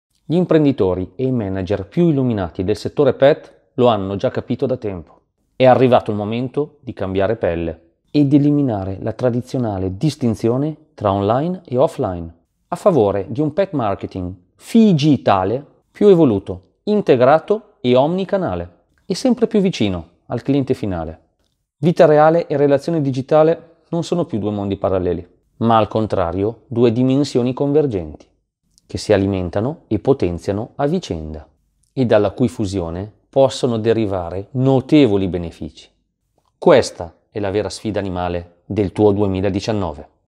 La vera sfida del 2019 è il FIGITALE
Il 2019 sarà l’anno del FIGITALE e segnerà la scomparsa della tradizionale distinzione tra online e offline, tra fisico e digitale, tra reale e virtuale. Scopri i 4 benefici a 4 zampe che la tua “azienda animale” non può permettersi di perdere!